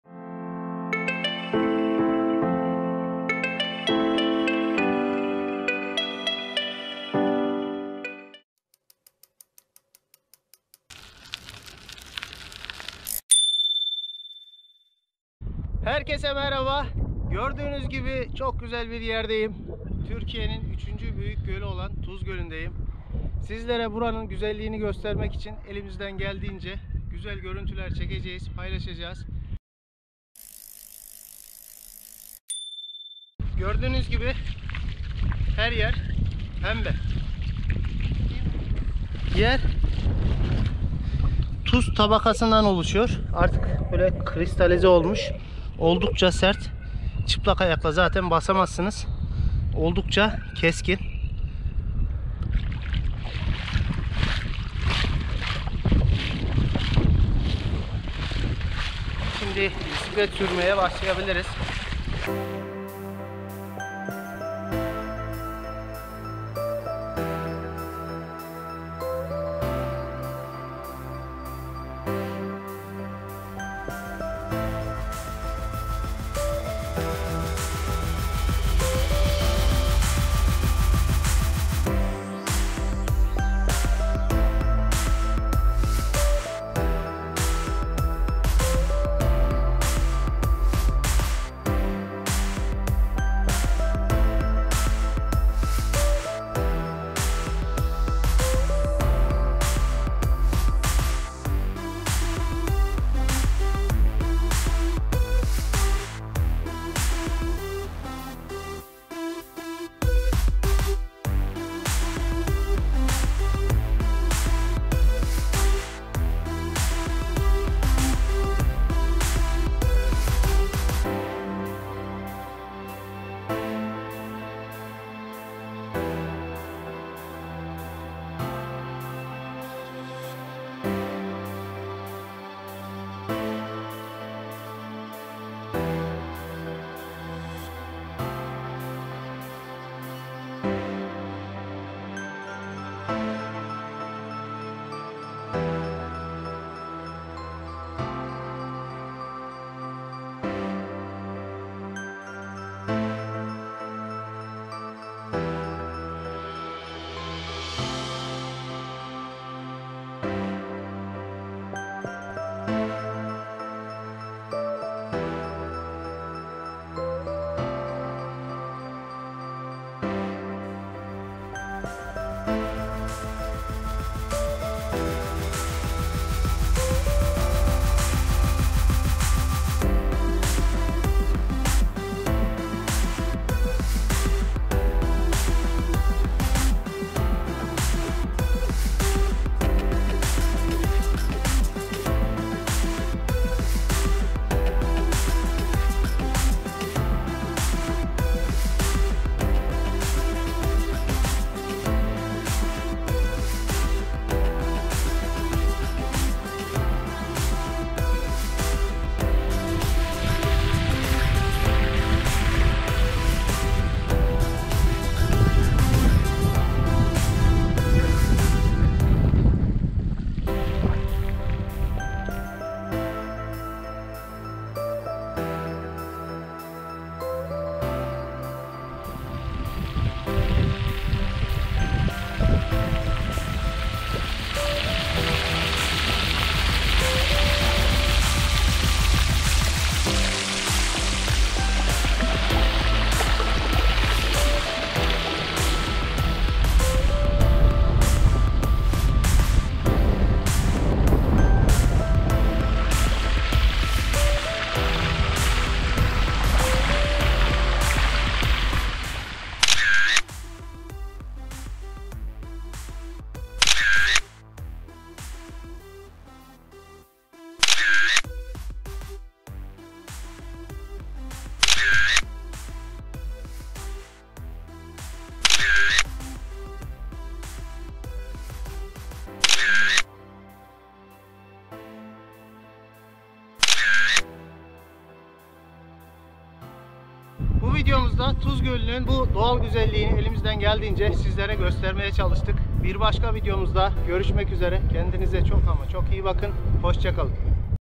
Tuz Gölü/Tuz Gölü Bisiklet Gezisi/Salt Lake Turkey/Pembe Göl
Merhaba, Türkiye'nin 3. Büyük Gölü olan Tuz Gölü'nde bisiklet sürdük ve bu doğal güzelliği sizin için ölümsüzleştirdik. İyi Seyirler...
Lütfen destek için videoyu beğenmeyi unutmayın... Teşekkürler...